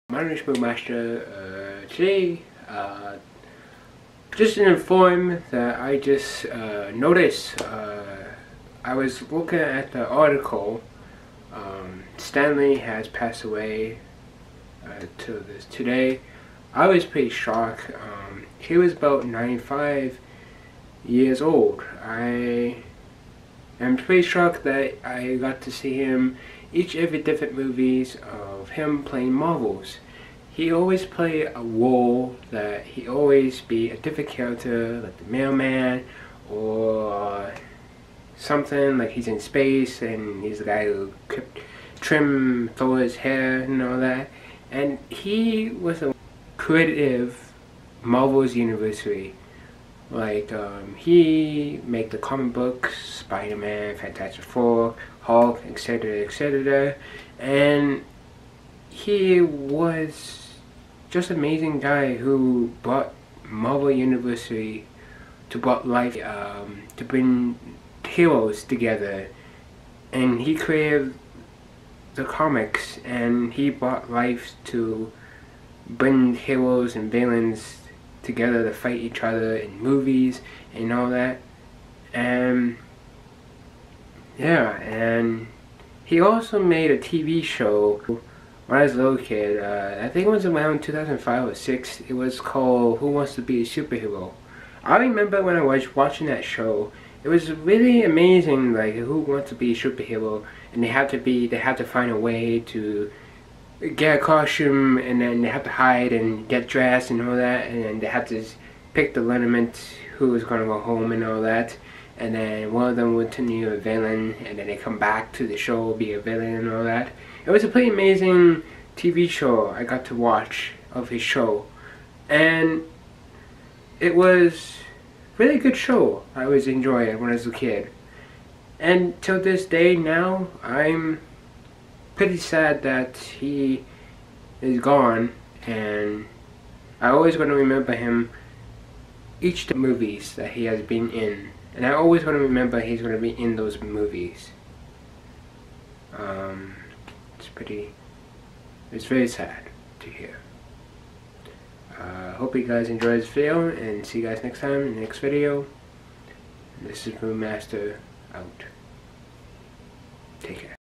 Stan lee has pass away :(
Stan Lee pass away one of the greatest comic book creditors of the Marvel University. I seen him every film in the superhero movies. Rest In Peace Stan Lee. ❤😢